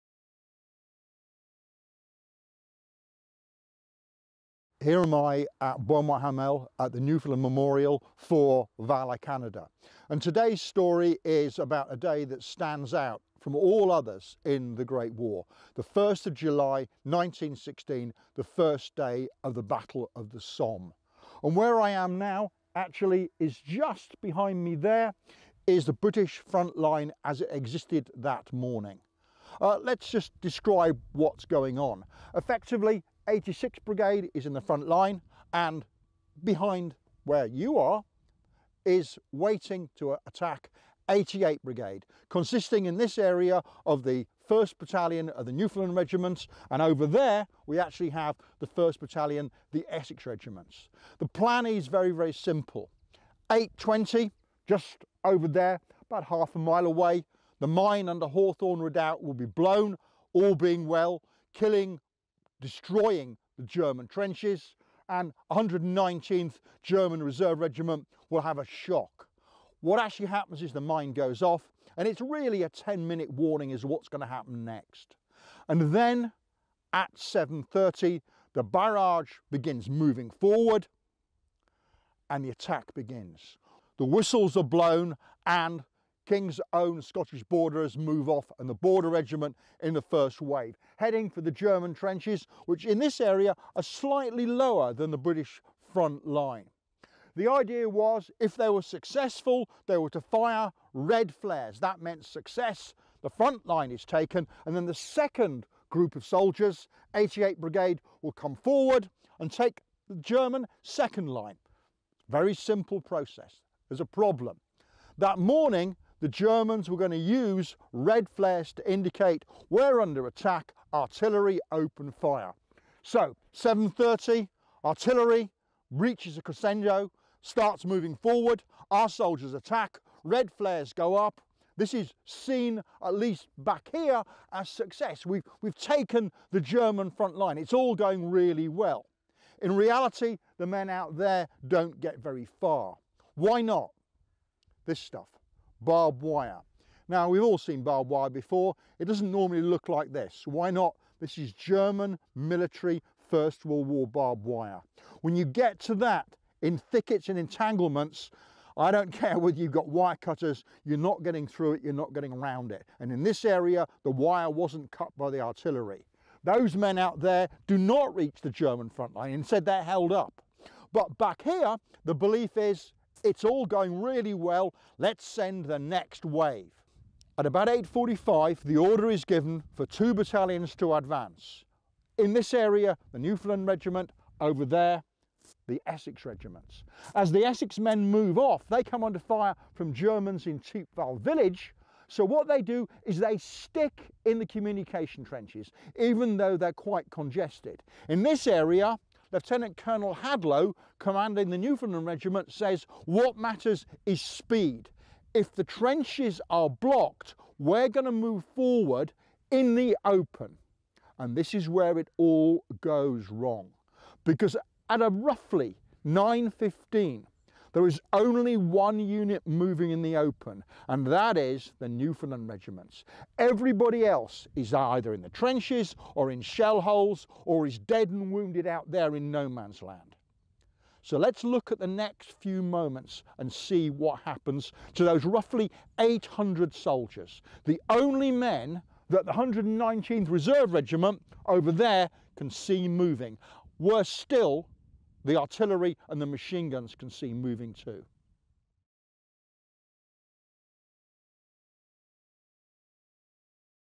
Beaumont Hamel - Newfoundlanders on the Somme (Pt. 1 of 2)
Andy Robertshaw describes Beaumont Hamel, a July 1916 battle on the Somme that involved a battalion of Newfoundlanders in July 1916. Part 1 of 2.
This video is part of the "Canada's FWW Battlefields" series.

Educators,
For a lesson plan that is paired with this vignette/playlist, please click this PDF